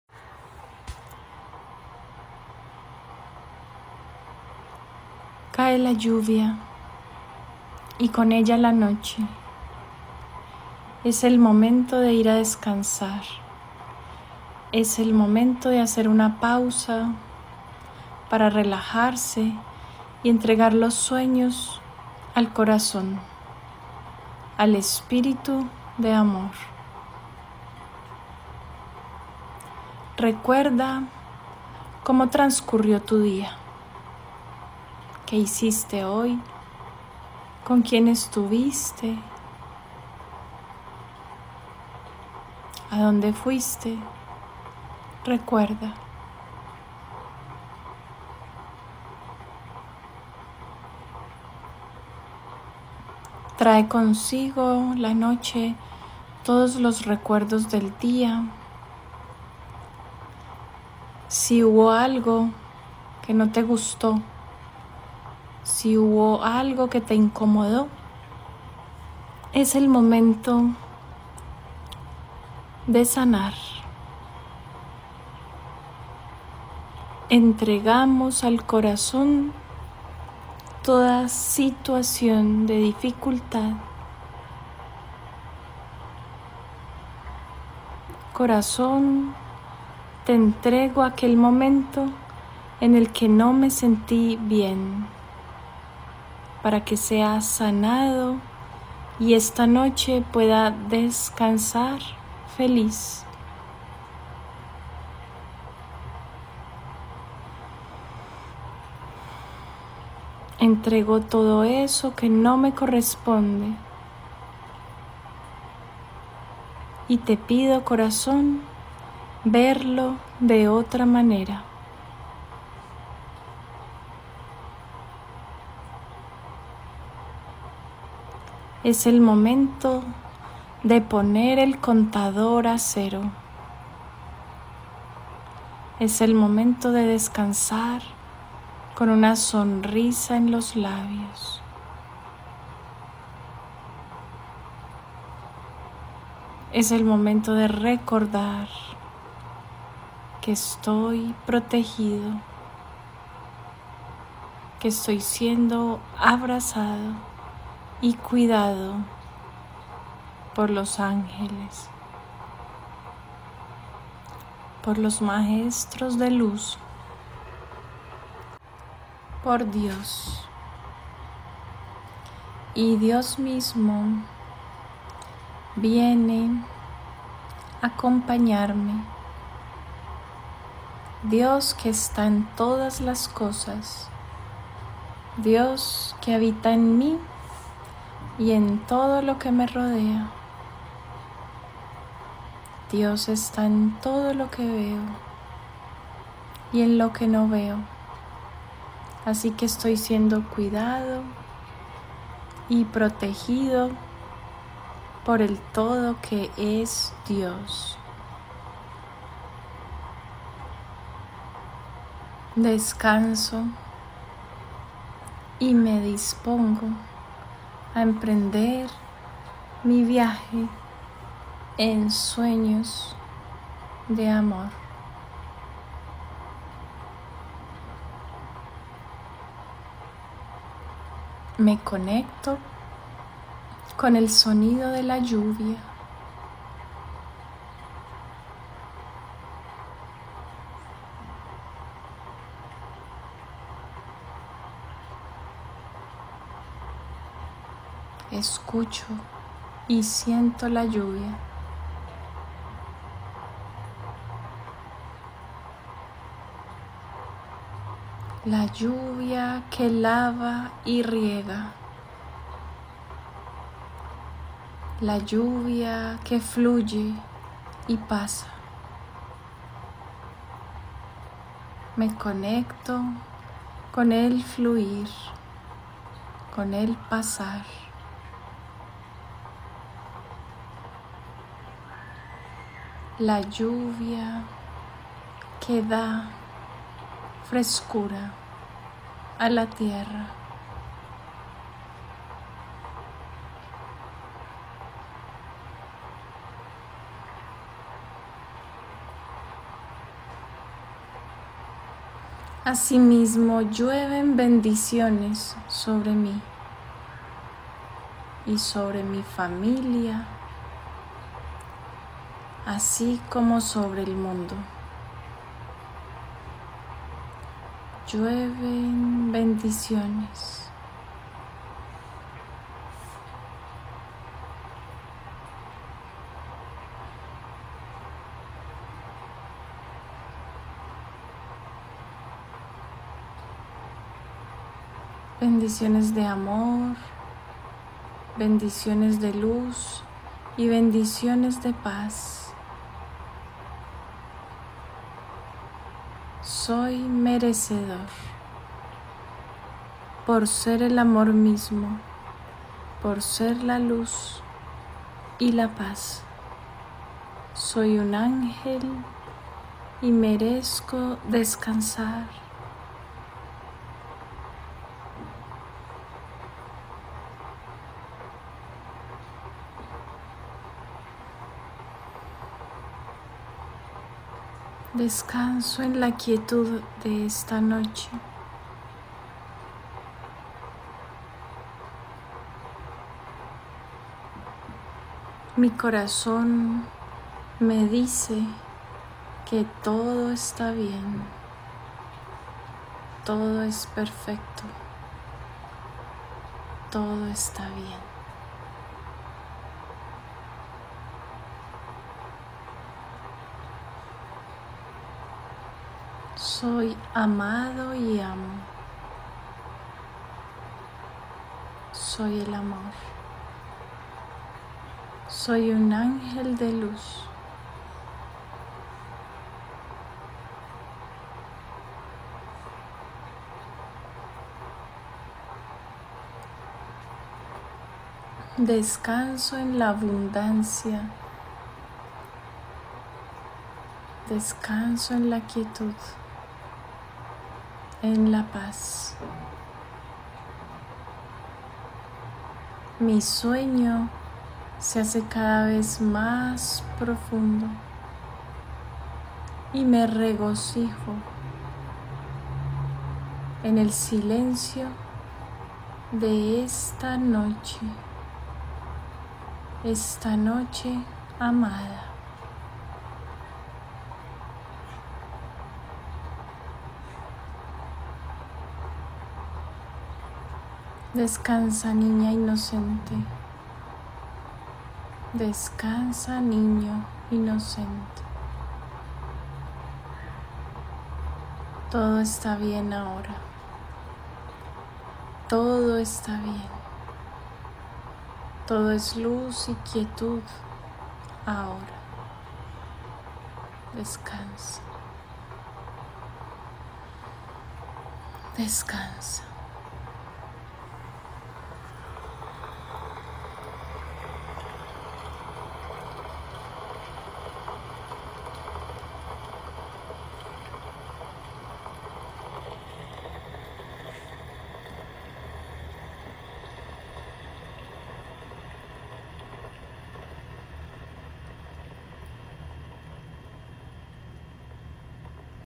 Pequeña meditación nocturna 4. Andrea Díaz
Al caer la noche y antes de dormir, medita y sueña feliz
Un deseo de Andrea Díaz El Despertar para tu Niñ@ interior